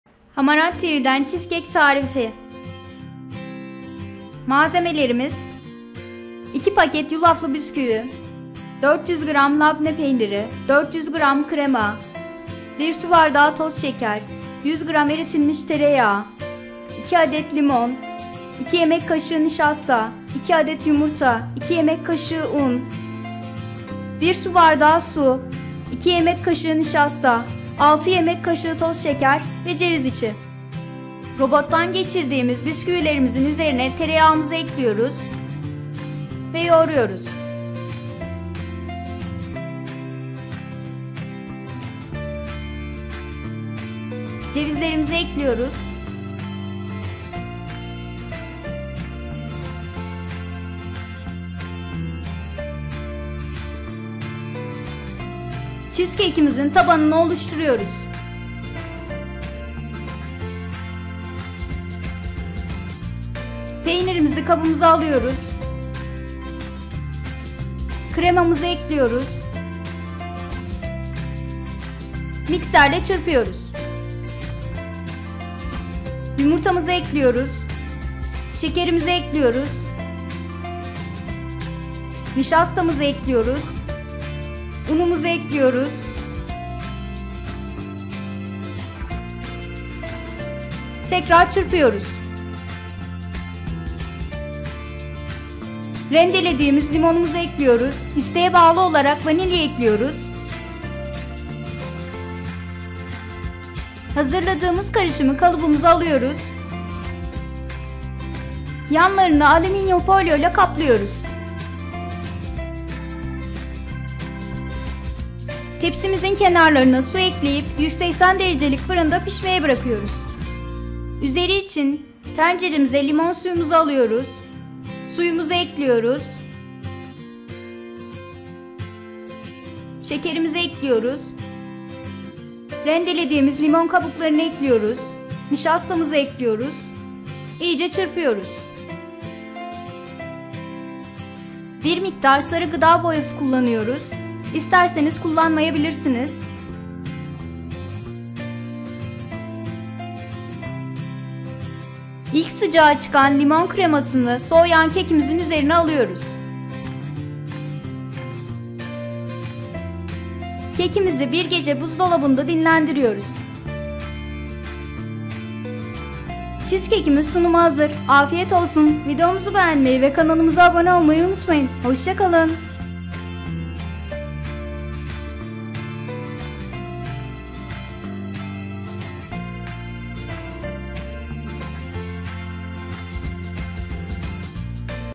Cheesecake Tarifi ve Malzemeleri
Cheesecake tarifi için hazırladığımız bu video ile cheesecake nasıl yapılır ve hangi malzemelerin kullanıldığını öğrenebilirsiniz.
Tarifi bizimle paylaşan Elif Karayılanoğlu hanıma teşekkür ederiz.

Cheesecake, limonunun yer aldığı her tarif gibi bu tarifimiz de oldukça lezzetli bir tariftir. Limonun müthiş aromasıyla lezzetine lezzet katılan cheesecake yiyen herkesi kendine hayran bırakacak, bir dilim daha istetecektir. Yapımı için videomuzdan yararlanabilirsiniz.

Videonun porsiyon miktarı;
Kaç Kişilik : 8
Hazırlama : 40 dk.
Pişirme : 25 dk.
Evde tarifi hazırleken, kişi sayısına göre malzemeyi arttırabilir yada azaltabilirsiniz.

Malzemeler
2 paket yulaflı bisküvi
400 gr labne peyniri
400 gr krema
1 su bardağı toz şeker
100 gr eritilmiş tereyağı
2 adet limon
2 yemek kaşığı nişasta
2 adet yumurta
2 yemek kaşığı un
1 su bardağı su
2 yemek kaşığı nişasta
6 yemek kaşığı toz şeker
Ceviz içi

Hazırlanışı
Cheesecake için bisküvileri robottan geçirip üzerine tereyağı ekleyip yoğurun. Cevizi ekleyip yoğurmaya devam edin. Cheesecake yapacağınız kalıbın tabanına hazırladığınız harcı yerleştirin. Labne peynirini kaba alın. Kremayı ekleyip mikserle çırpın. Yumurtayı, şekeri, nişastayı ve unu ekleyip mikserle çırpmaya devam edin. Rendelediğiniz limon ve isteğe bağlı vanilya ekleyip karıştırın. Hazırladığınız karışımı kalıba dökün. Yanlarını alüminyum folyo ile kaplayın. Tepsinin kenarlarına su ekleyerek 180 derecelik fırında pişmeye bırakın. Üzeri için tencereye limon suyunu, suyu, şekeri, rendelediğiniz limon kabuğu ve nişastayı ekleyip iyice çırpın. Bir miktar sarı gıda boyası kullanıp kaynatın. İlk sıcaklığını atan limon kremasını soğuyan kekin üzerine dökün. Keki bir gece buzdolabında dinlendirin. Afiyet olsun.

Servisi/Sunumu
Cheesecake hazırlandıktan sonra buzdolabında mutlaka bir gün bekletilmelidir. Servis edileceği zaman üçgen yani pizza dilimi şeklinde kesilip çayın yanında ikram edilebilir.

Tarifinin Püf Noktaları
Cheesecake yapımında önemli nokta tatlının dolapta bekletme süresidir. Her aşamayı tamamen kurallara uygun şekilde tamamlarsanız tatlınız güzel olacaktır.

Cheesecake Besin Değeleri
  1 Porsiyon Miktarı : 142 g
  Kilojul : 1,62 kj
  Kaloriler : 388 Kcal
  Protein : 8,88 g
  Karbonhidrat : 46,72 g
  Şeker : 37,91 g
  Yağ : 19,1 g
  Doymuş Yağ : 7,631 g
  Tekli Doymamış Yağ : 7,15 g
  Çoklu Doymamış Yağ : 3,242 g
  Kolesterol : 67 mg
  Fiber : 0,7 g
  Sodyum : 378 mg
  Potasyum : 124 mg

Hamarat TV sosyal medyada takip edin